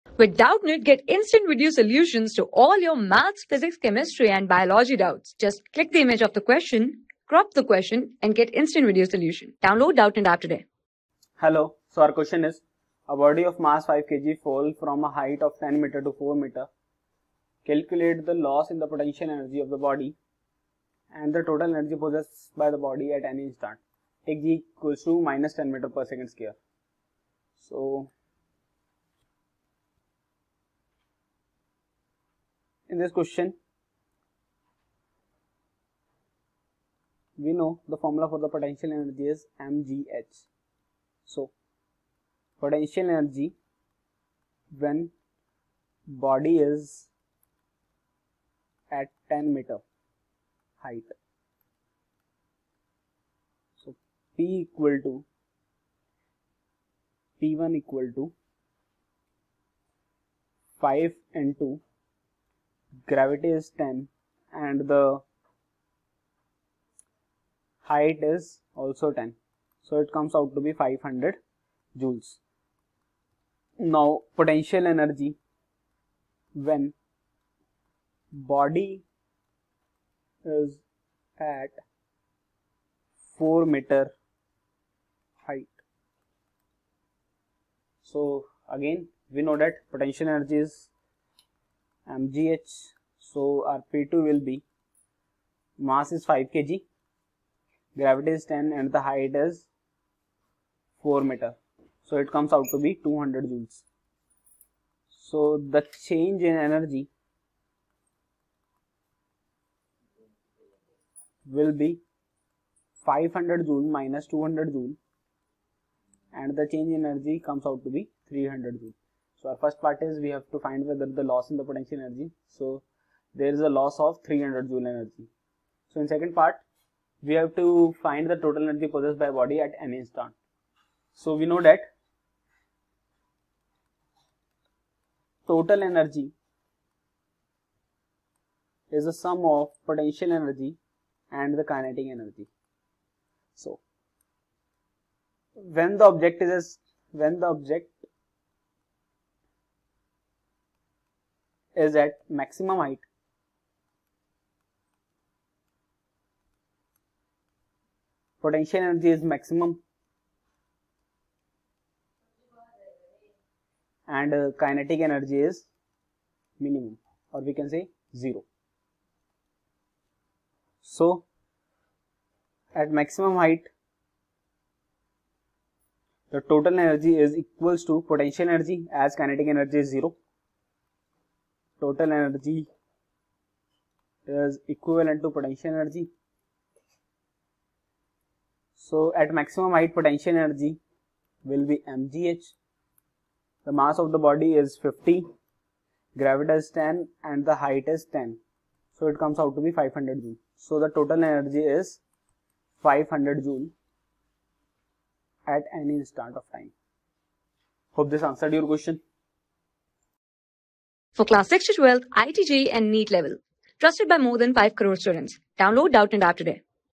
A body of mass 5 kg falls from a height of 10 m to 4 m. Calculate : (i) the loss in potential e...
A body of mass 5 kg falls from a height of 10 m to 4 m. Calculate : (i) the loss in potential energy of the body, and (ii) the total energy possessed by the body at any instant ? (Take -10 m s^(-2)
Class: 10
Subject: PHYSICS
Chapter:  WORK ENERGY AND  POWER
Board:ICSE

👉 Have Any Query? Ask Us.